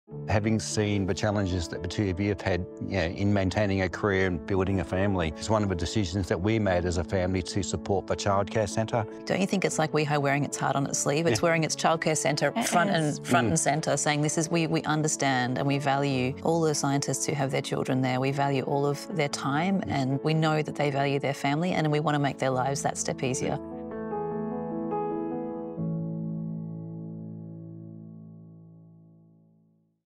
Supporting bright science careers
Our medical researchers dedicate their lives to keeping us healthy. Discover how investing in the brightest minds of science – from early career support to balancing research and family life – can have far-reaching impacts with donor, John.